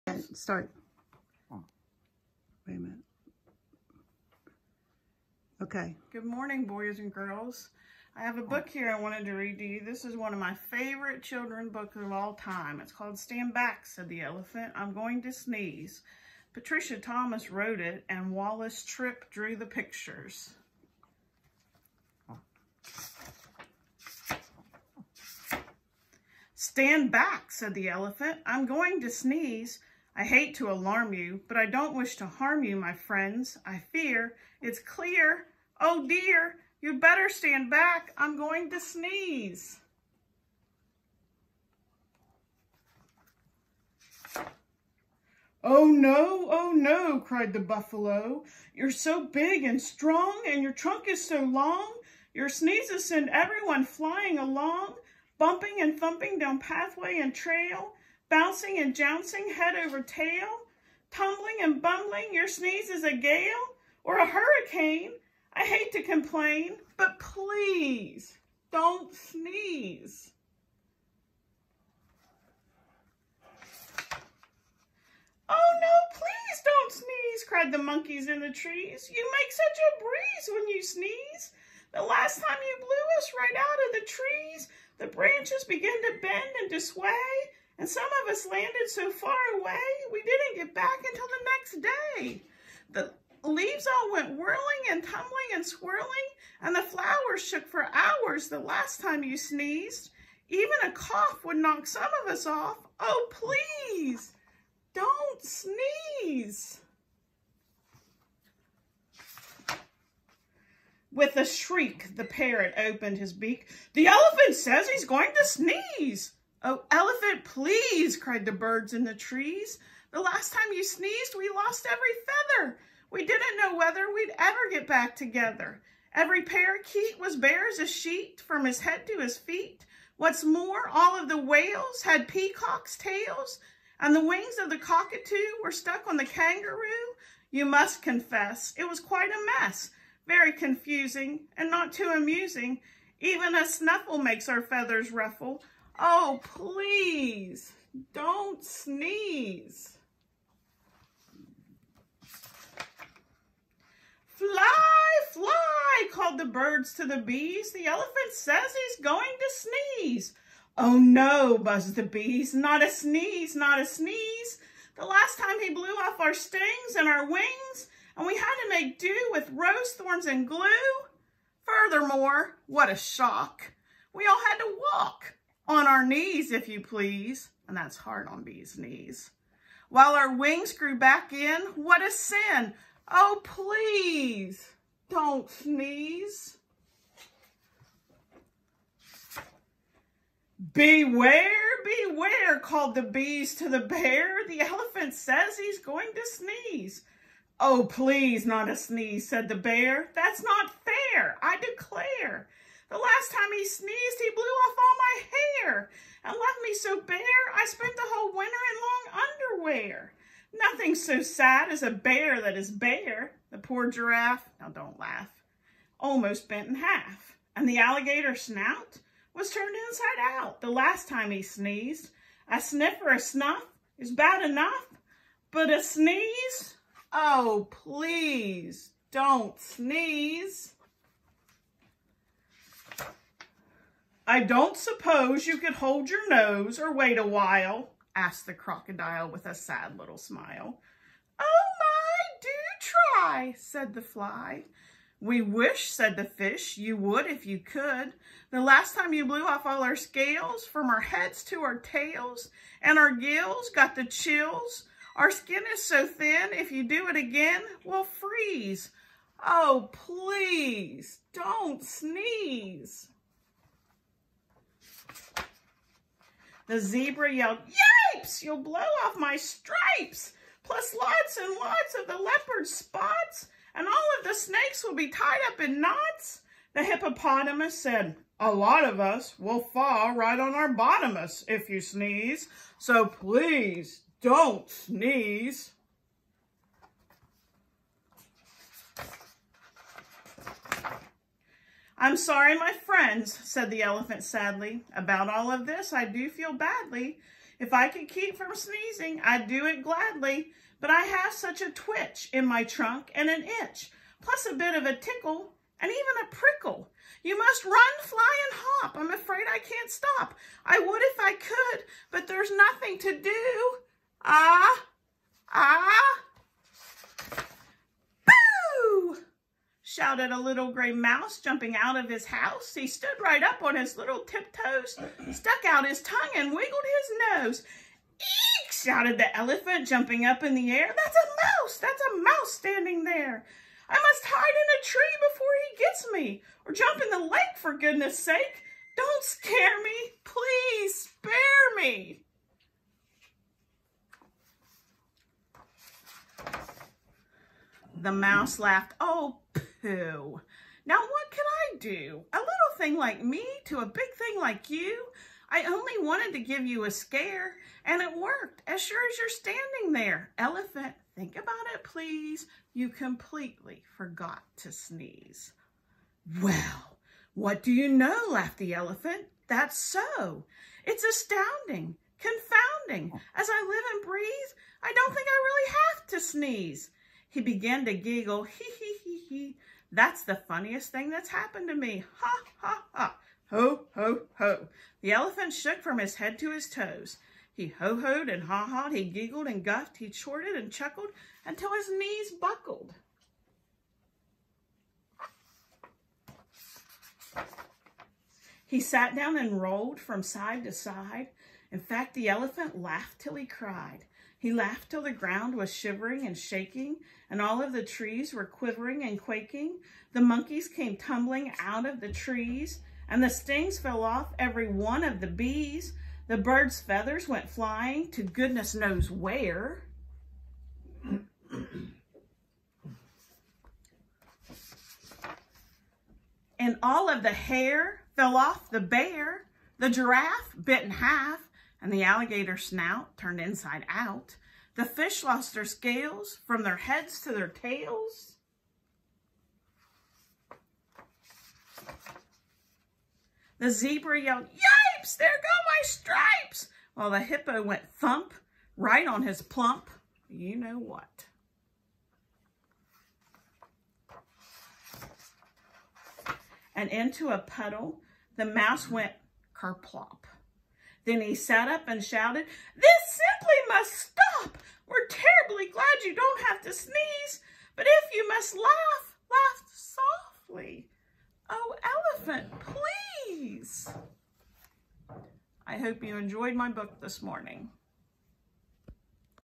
“Stand back!" said the elephant. "I am going to sneeze!” Read aloud
4th-grade teacher at DCES Rhonda Powell reading “Stand back!" said the elephant. "I am going to sneeze!”